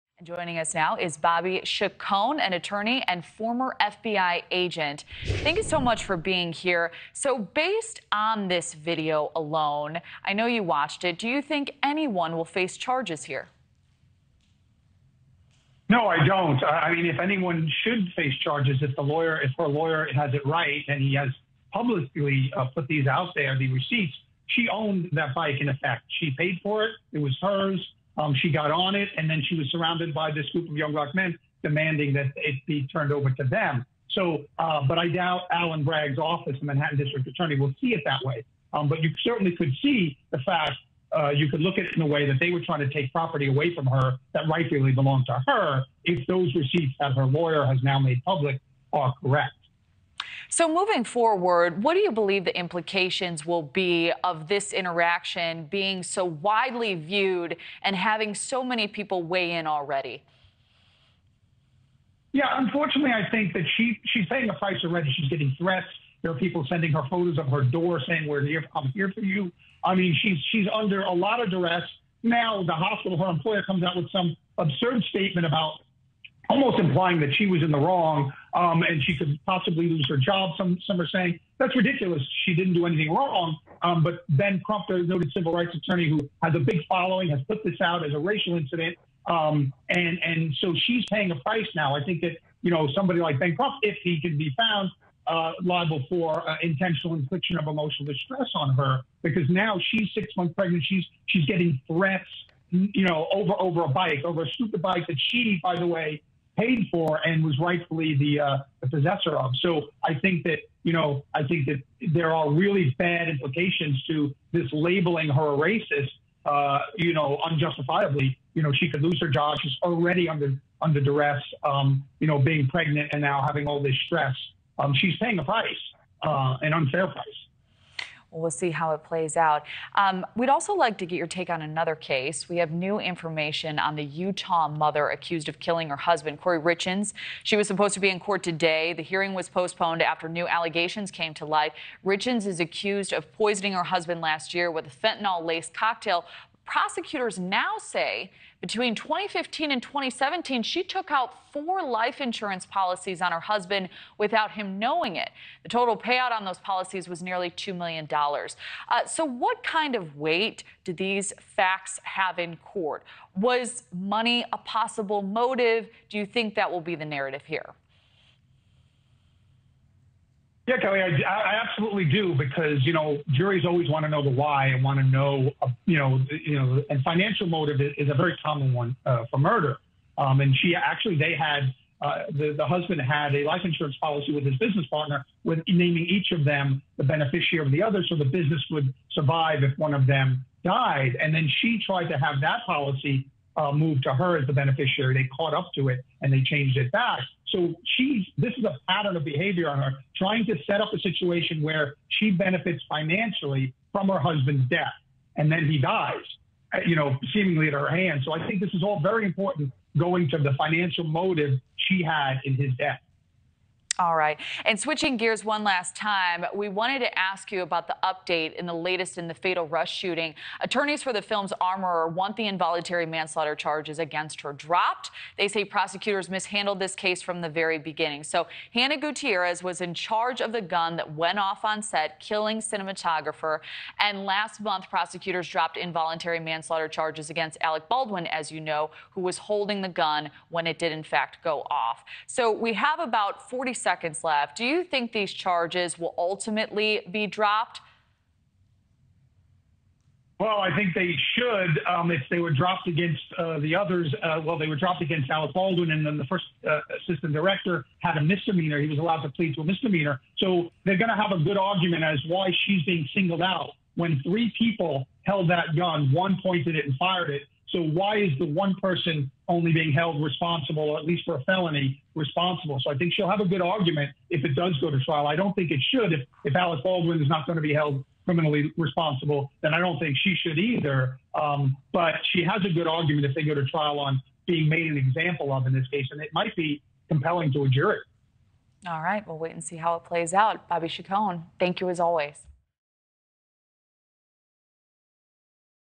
Bobby Chacon NewsNation 19May23 SD 480p
Bobby Chacon covers the latest on the viral video of the NYC nurse in a confrontation over a rental bike, the Utah wife accused of murdering her husband by fentanyl poisoning and the Rust movie set armorer as the lone person remaining to go to trial over the tragic shooting death of cinematographer Halyna Hutchins.